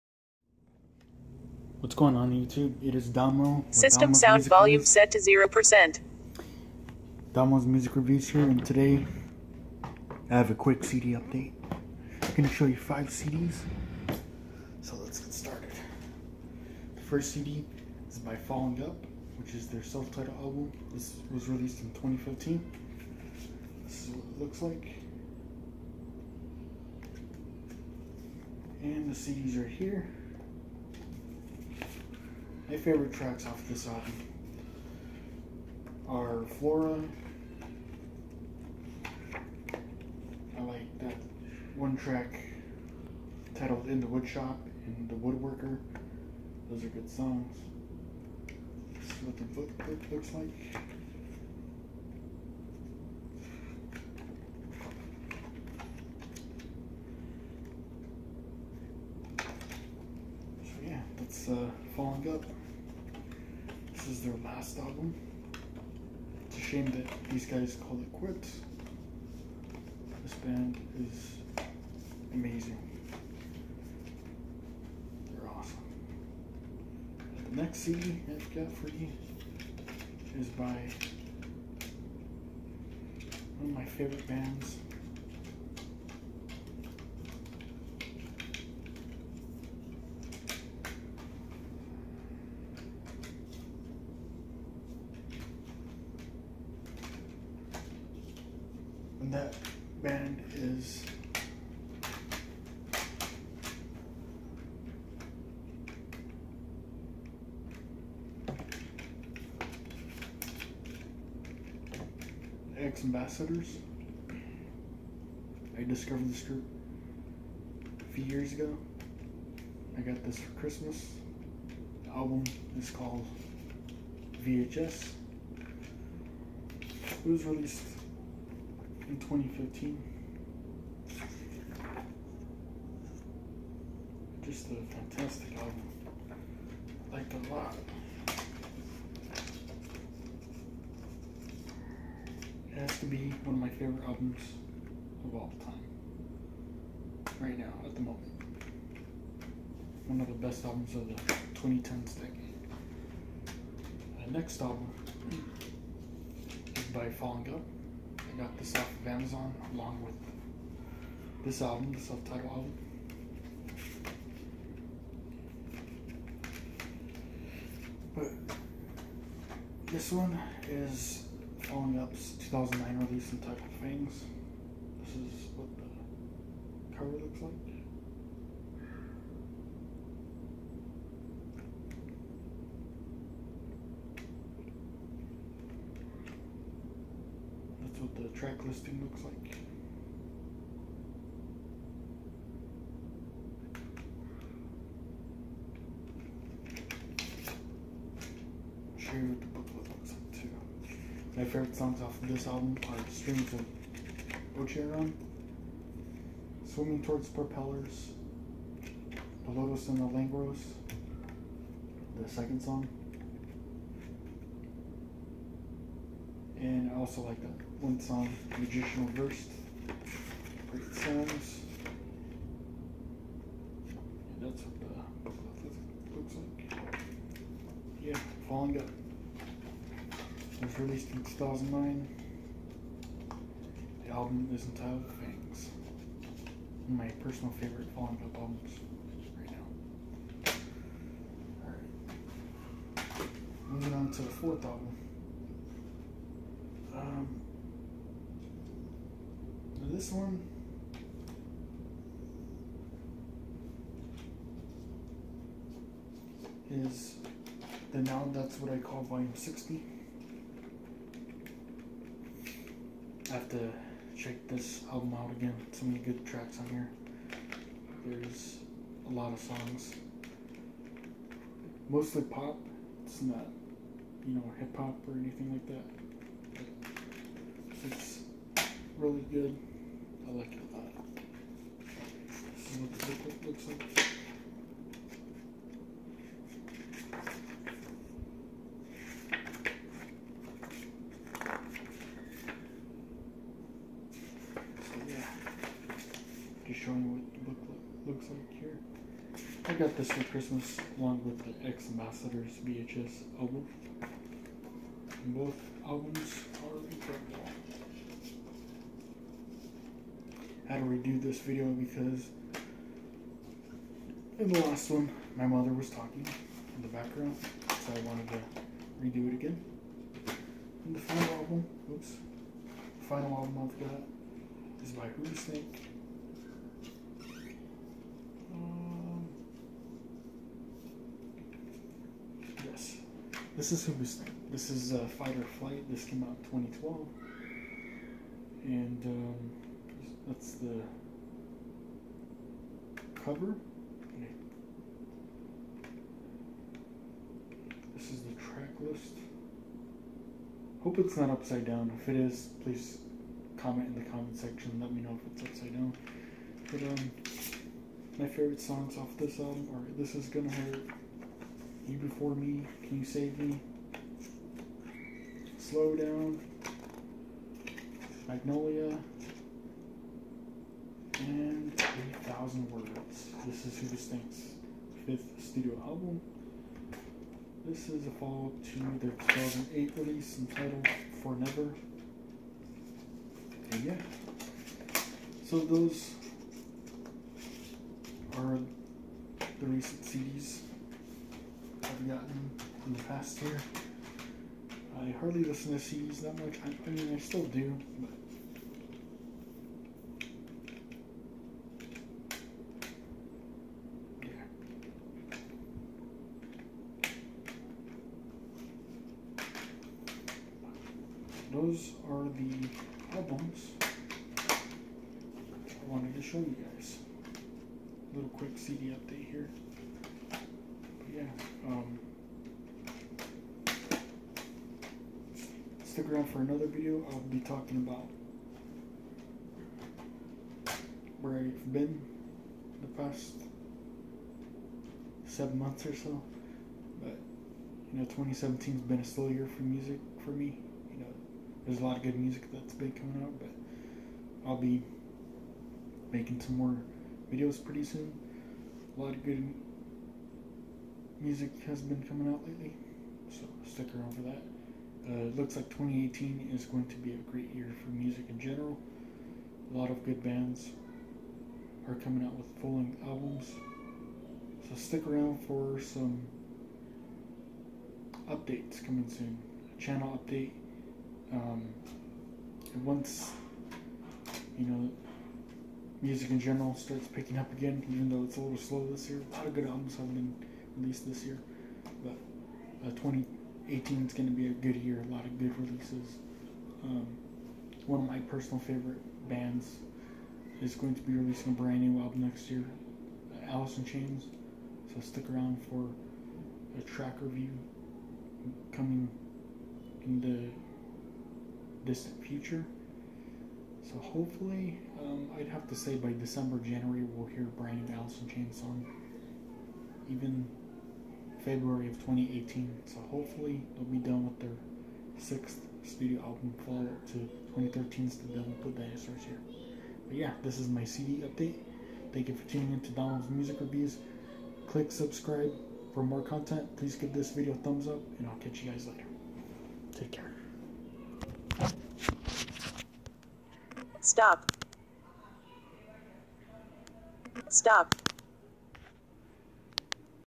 A Quick CD Update!
In this video, you show you the recent five CD's that I got in the past year.